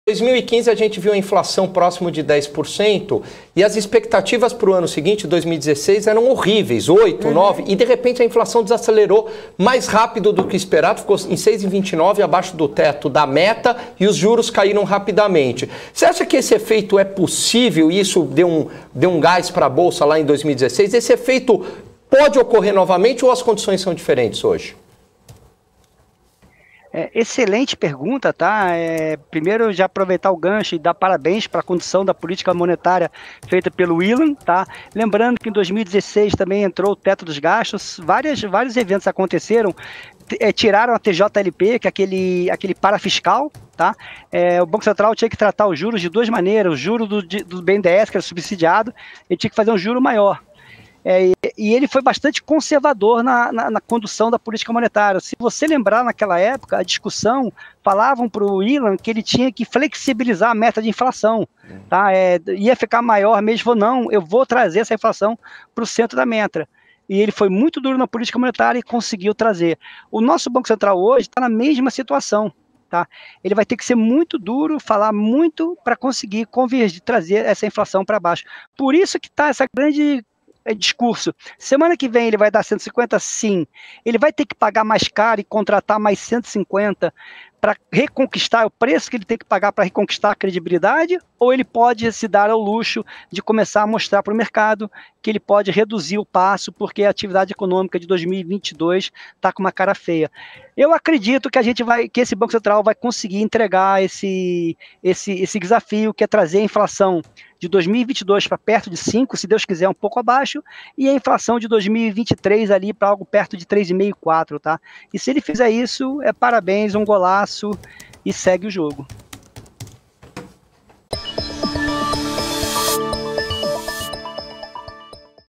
O efeito de queda da inflação em 2016 pode ocorrer hoje?
Roberto Motte, da Genial, analisa a situação de queda da inflação em 2016, se pode acontecer para o próximo ano.